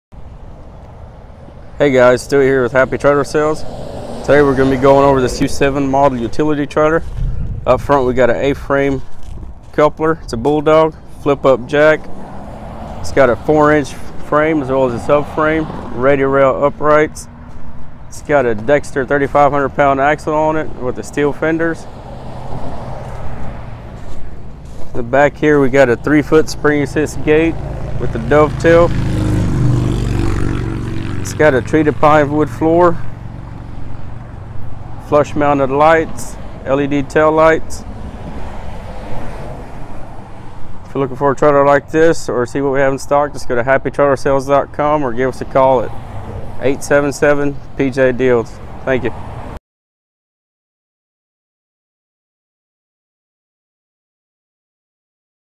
2019 PJ Utility Trailer Overview by Happy Trailer Sales
In this video, Stewie walks us through some of the features found on this new 2019 PJ 77"x12' Utiliy Trailer [U7] at our Powderly, TX dealership location. Give us a call at 1 (877) PJ DEALS or visit us